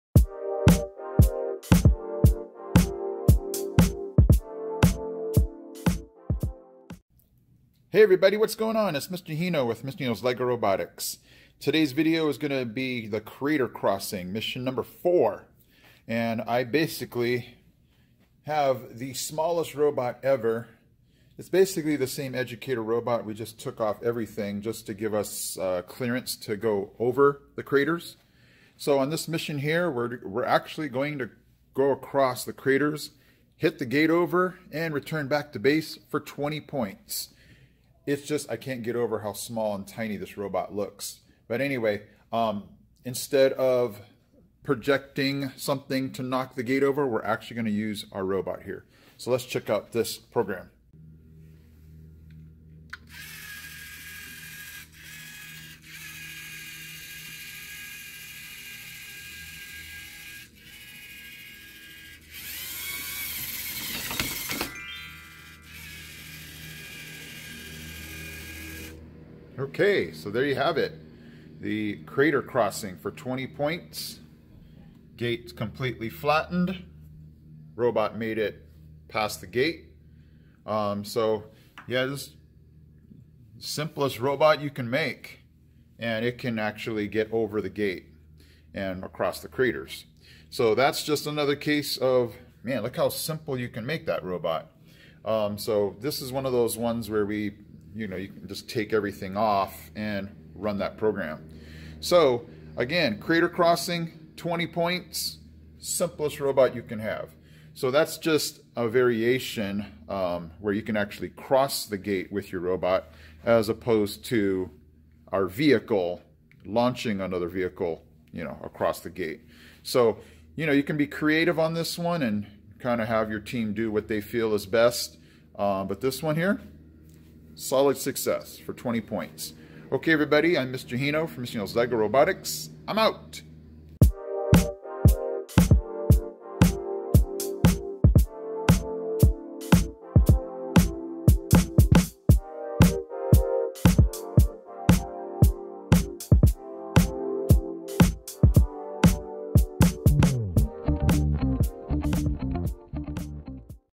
"2018 Into Orbit: Completing The Crater Crossing Mission M04"
Hey guys!!!  This video shows the "Crater Crossing" Mission (M04) completed for 20 points by having the robot cross and flatten the gate.  Hope you enjoy!!!